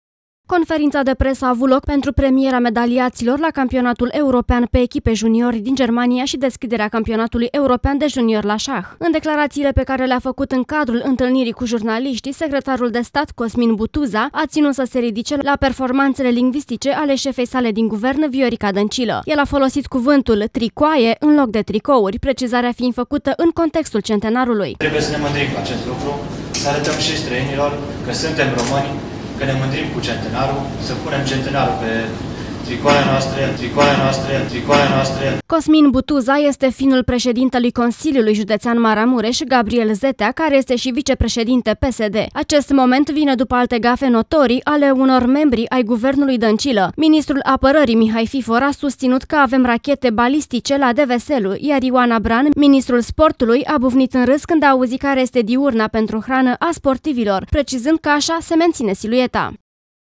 TRICOAIELE CENTENARULUI
Nu doar premierul Viorica Dăncilă reușește să chinuie limba română și să stârnească glume și comentarii acide, ci și unii dintre subordonații săi din Guvern. Cea mai recentă perlă linvistică aparține lui Cosmin Butuza, secretar de stat în cadrul Ministerului Tineretului şi Sportului. Într-o conferință de presă susţinută la Federaţia de Şah, el a folosit cuvântul „tricoaie” în loc de „tricouri", precizarea fiind făcută în contextul Centenarului.